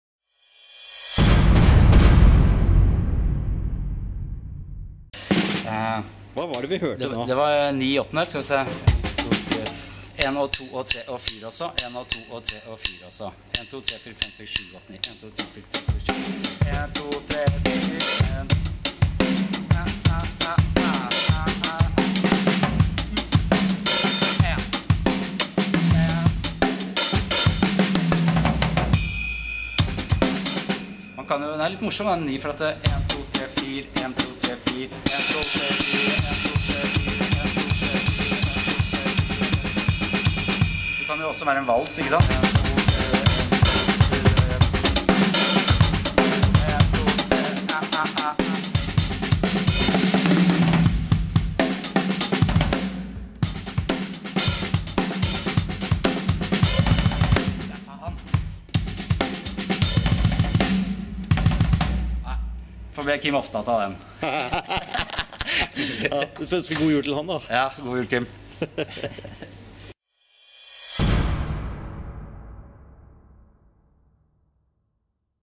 Jarle Vespestad spiller 9/8-dels takt.
Jarle Vespestad viser en typisk Farmers Markets groove i 9/8. Den deles opp i både 2+2+2+3 og 3+3+3. Til slutt møter Jarle en umulig figur som han spiller over til en god kollega :-)